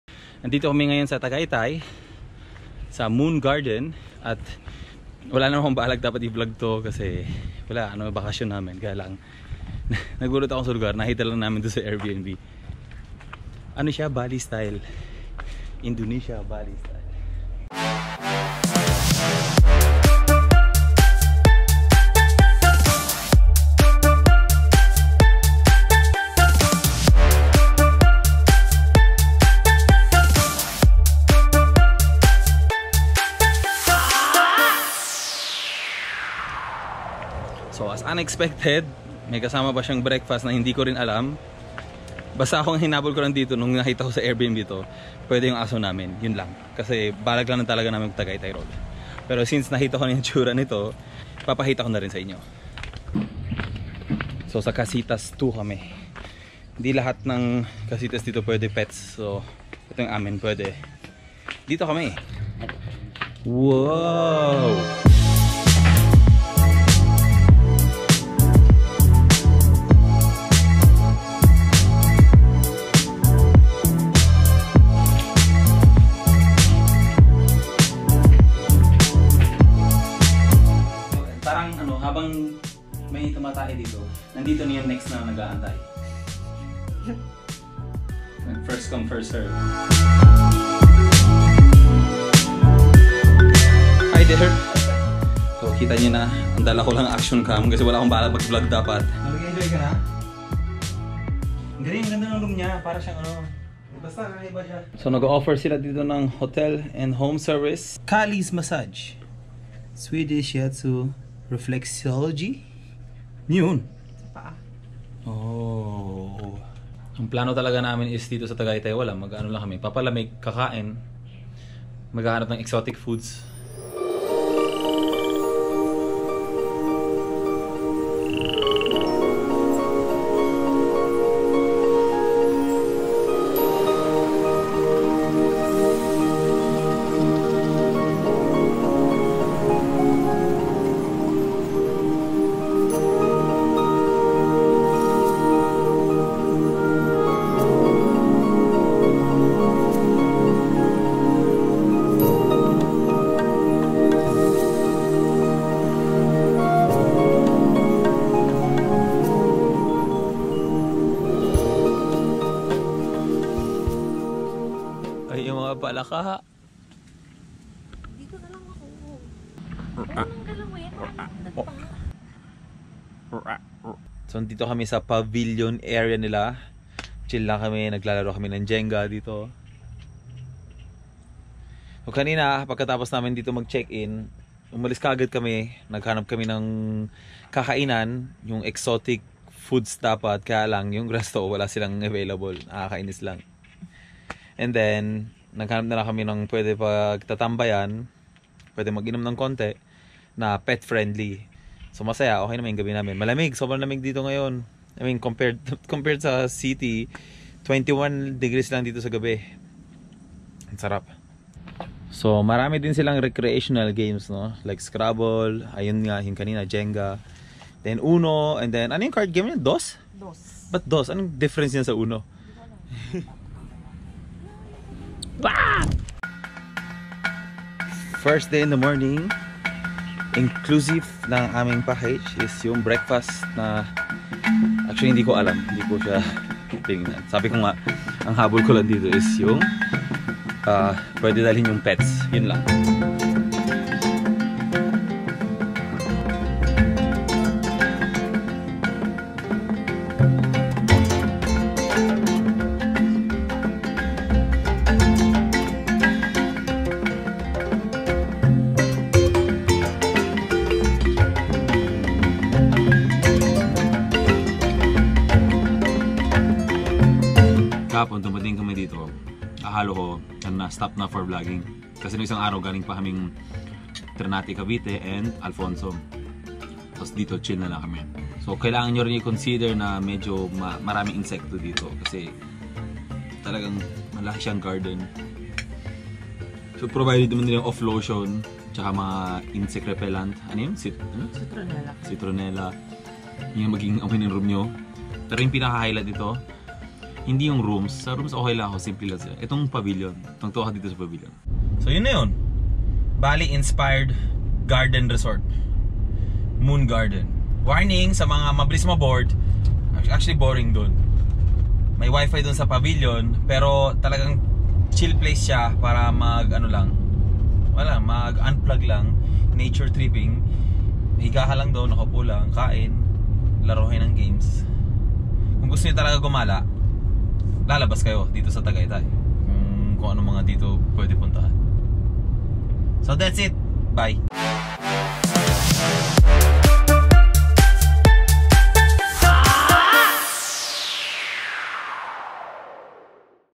BALI INDONESIA inspired garden in TAGAYTAY (Pet friendly)
Check out this Airbnb rental, BALI INDONESIA inspired at the Moon Garden, Tagaytay City

Watch the vlog for more details.

Like my FB Page: XIAN STOI